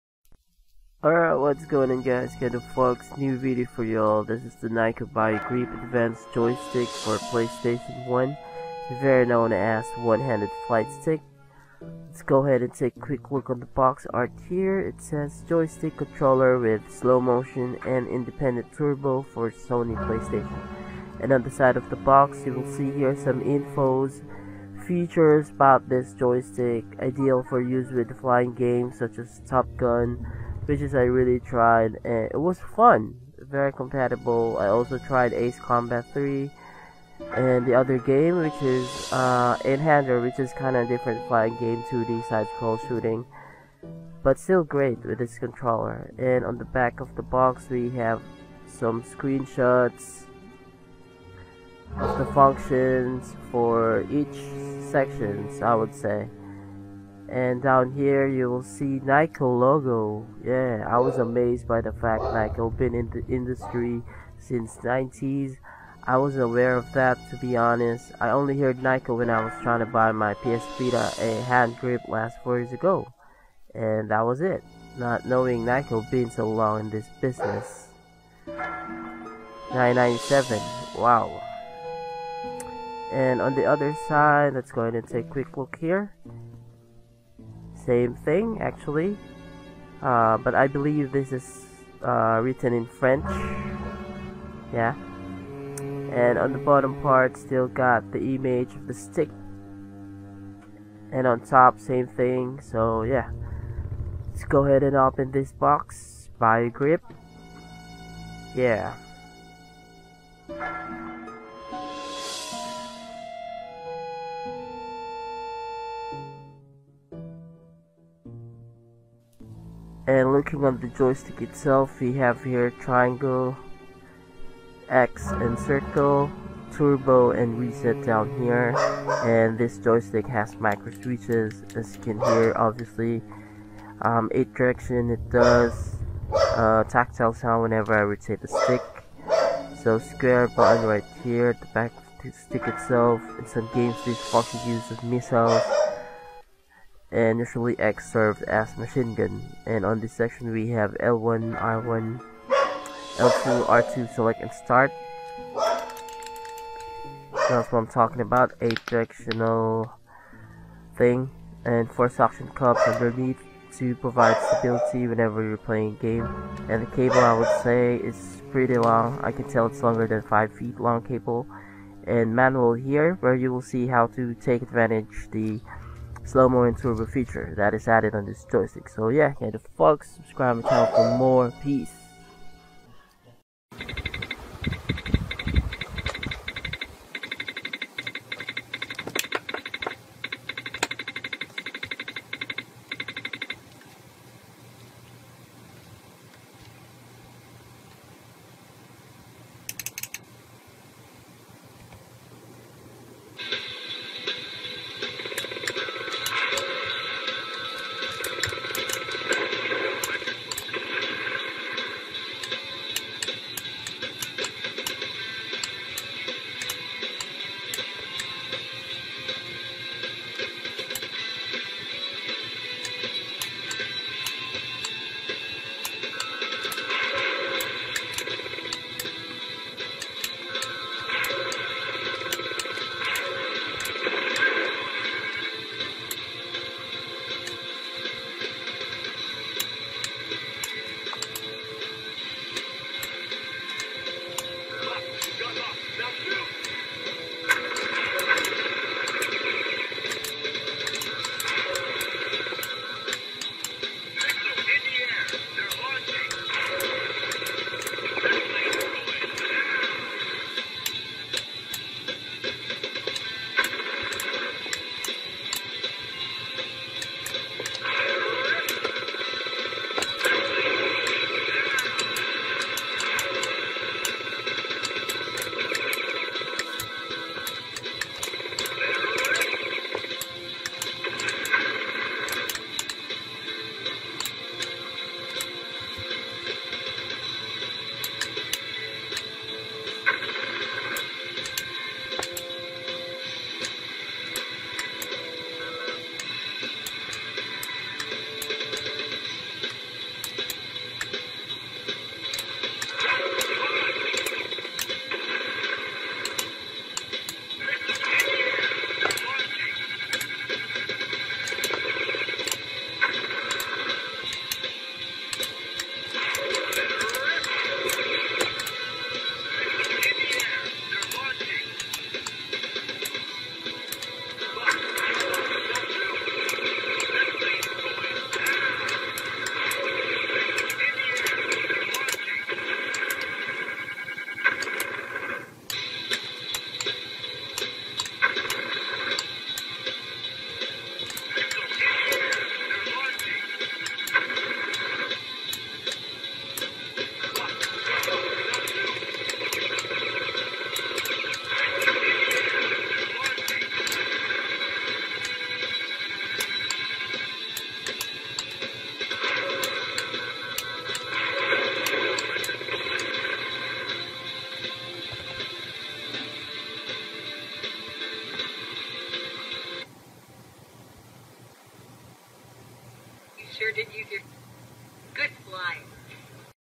Playstation NYKO Bio Grip Advanced Joystick Unboxing/Gameplay
Unboxing Playstation NYKO Bio Grip Advanced Joystick + GAMEPLAY TEST!

I've uploaded this review originally sometime in January of 2018 this joystick/flightstick is very good and very clicky when firing missiles and rockets